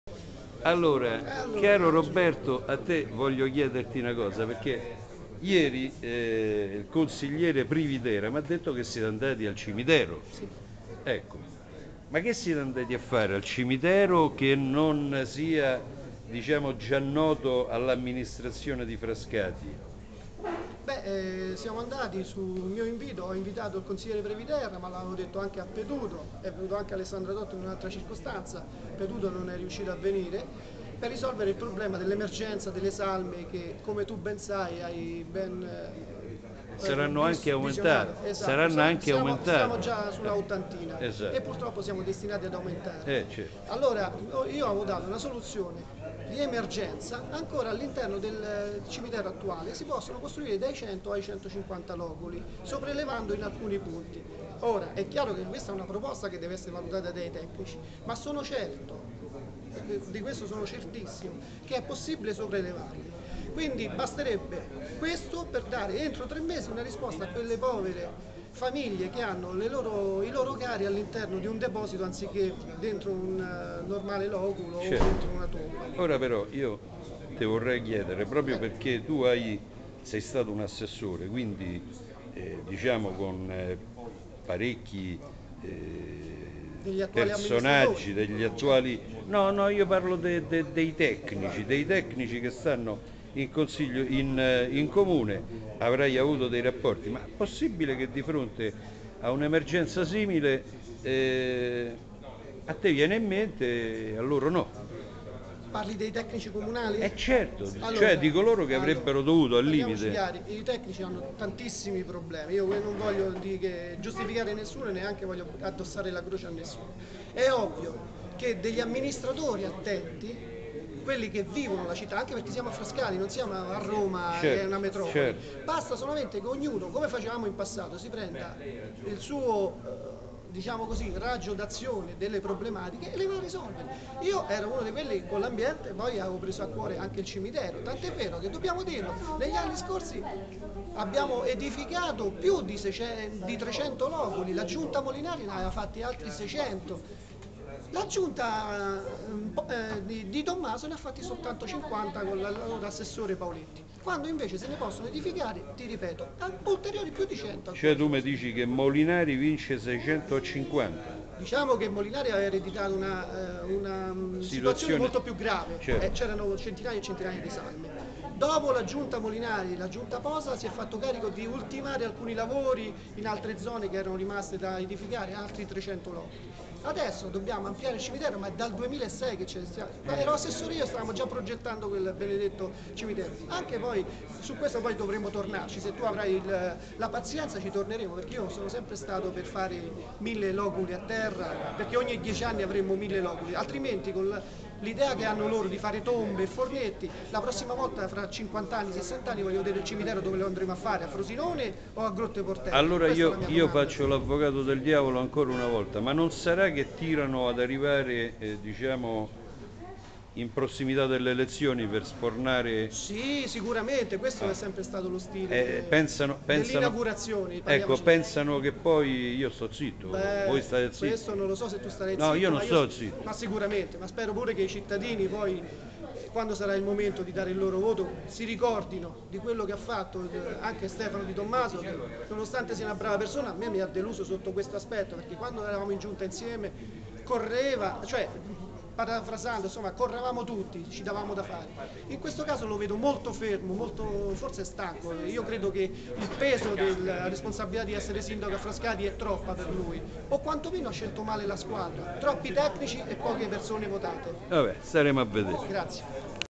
Progetto periferie 1 002 - CARO ROBERTO PARLAMI DEL CIMITERO
Accade che, in merito ai problemi cimiteriali, un ex assessore intraveda una soluzione che sedicenti attenti amministratori non vedono. E gli uffici tecnici neppure le segnalano loro.
E di Di Tommaso che mi dici? E' fermo. O l'impegno è troppo per lui o ha scelto male la squadra !